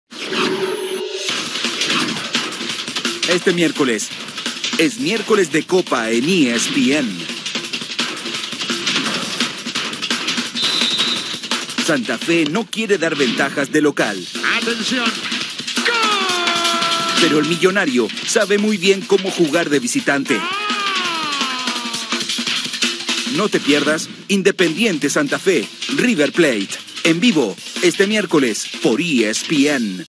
Promo ESPN | Santa Fe vs River Plate - Copa Libertadores 2021
Volvió la Copa Libertadores Temporada 2021

Promo ESPN

De vuelta en la fase de grupos de la máxima competición internacional de fútbol en Sudamérica, Independiente Santa Fe (COL) vs River Plate (ARG) se enfrentan en el grupo D.

Fecha 3 de la Conmebol Libertadores 2021.